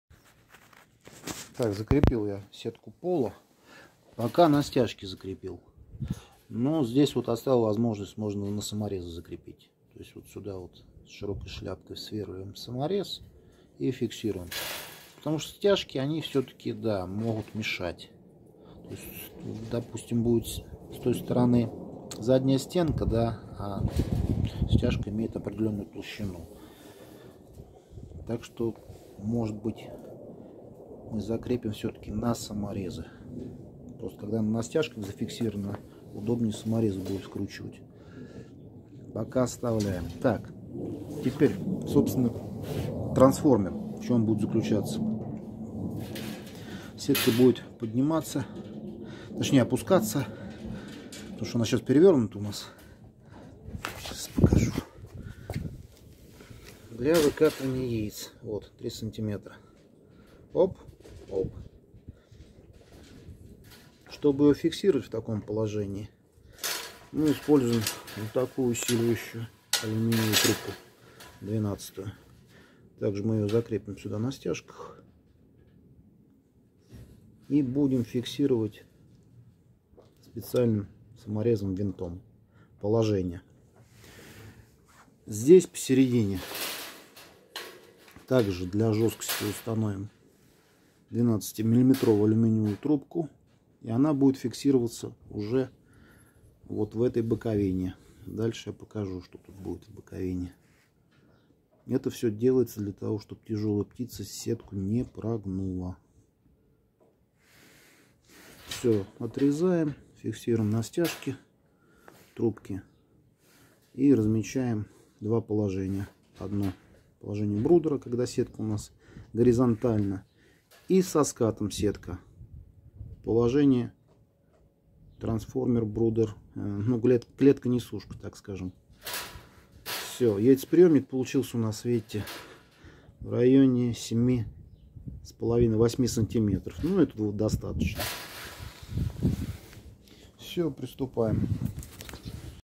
Правильный брудер "Lamba" vs "Ferra" (часть 6)
Крепим сетку пола брудера- трансформера пластиковыми стяжками.